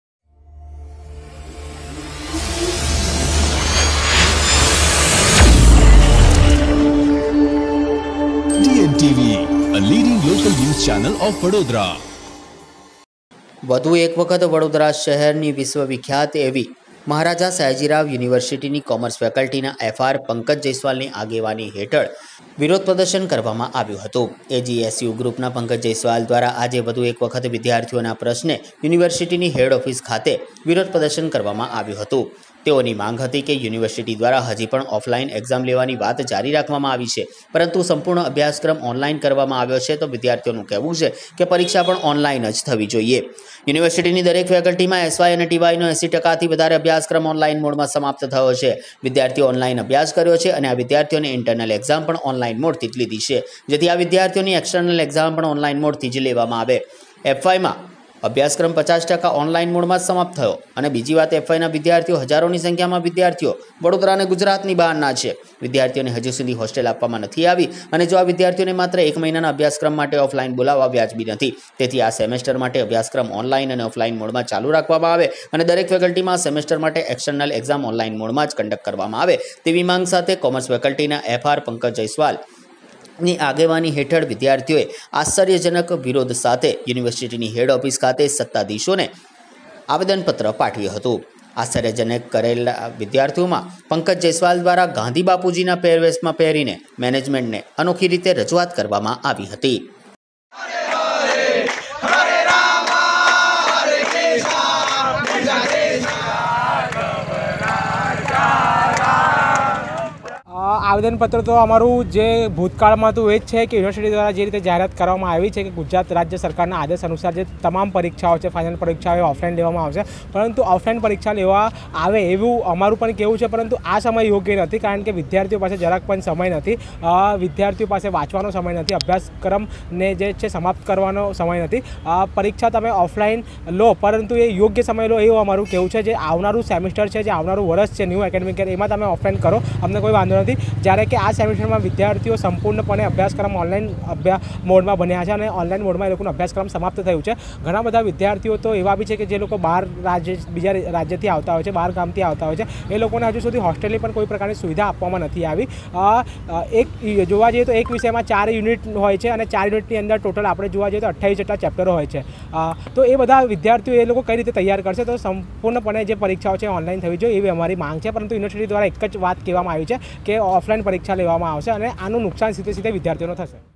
04-04-2022 | યુનિવર્સિટી ની હેડ ખાતે AGSU દ્વારા આશ્ચર્યજનક વિરોધ
આજ રોજ પણ યુનિવર્સિટી ની હેડ ખાતે All Gujarat Students Union ( AGSU ) દ્વારા આશ્ચર્યજનક વિરોધ કરવામાં આવશે પંકજ જેસવાલ ગાંધીબાપુ ના પહેરવેશ પહેરીને મેનેજમેન્ટને રજૂઆત કરી

Baroda AGSU Head Office Virodh